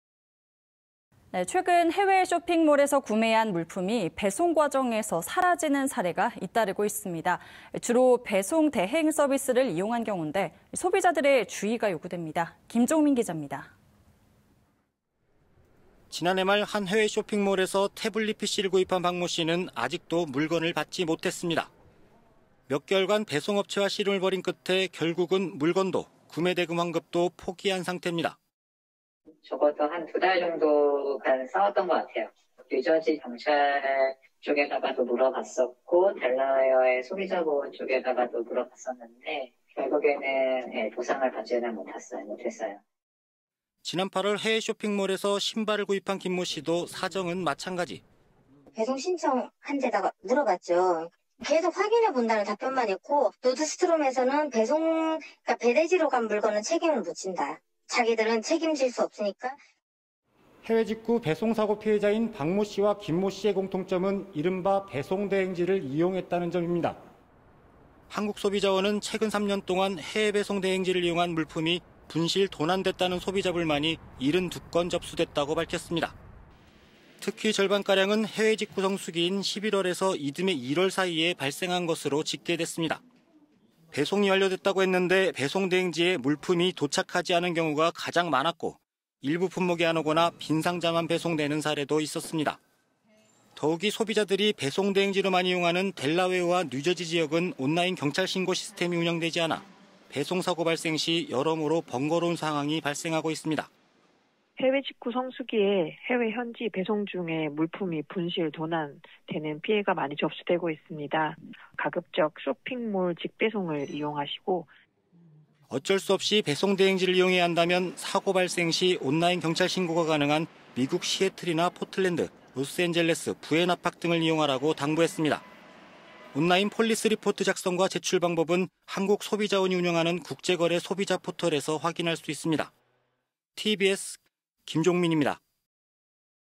해외직구 '배달사고' 주의보…배송대행 이용시 주의해야
최근 해외 쇼핑몰에서 구매한 물품이 배송 과정에서 사라지는 사례가 잇따르고 있습니다.

주로 배송 대행 서비스를 이용한 경우인데, 소비자들의 주의가 요구됩니다.

김종민 기자입니다.

▣ 뉴스 제보